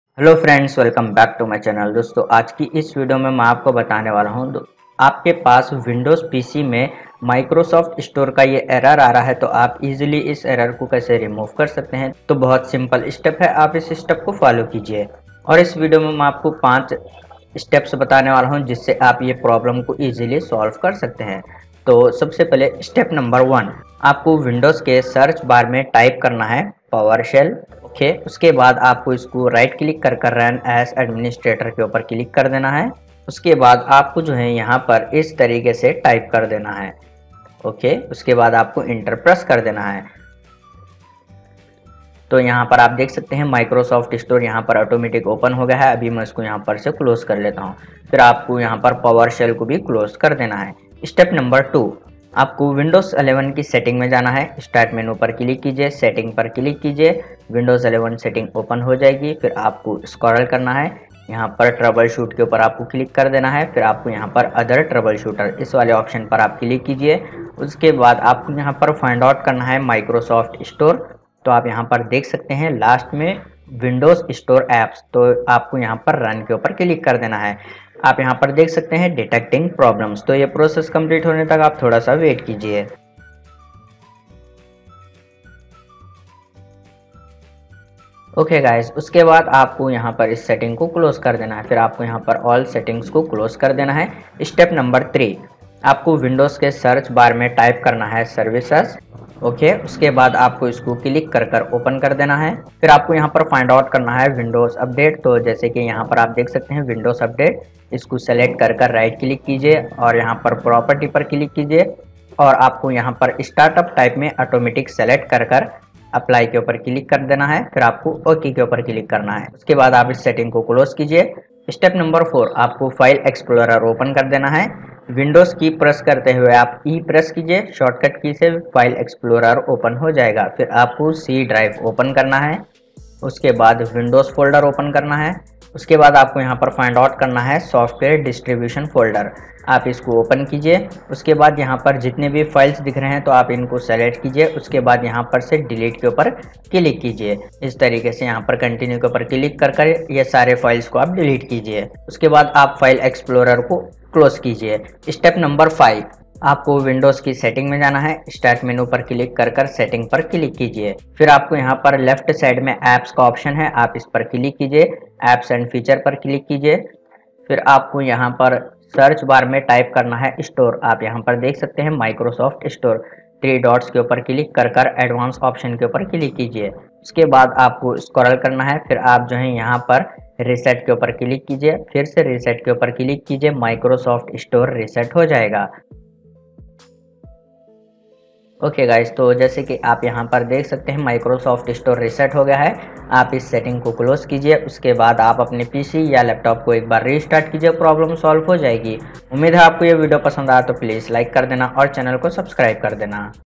How to Fix Microsoft Store Error Code 0x00000193 in Windows 11
How to Fix Try that again something happened on our end. Waiting a bit might help error code 0x00000193 in Microsoft store in windows 11/10

Video Highlights:
00:00 Intro
00:23 Powershell
00:52 Windows 11 Setting
01:34 Windows Update
02:02 C Drive
02:35 Reset Microsoft Store
03:15 Outro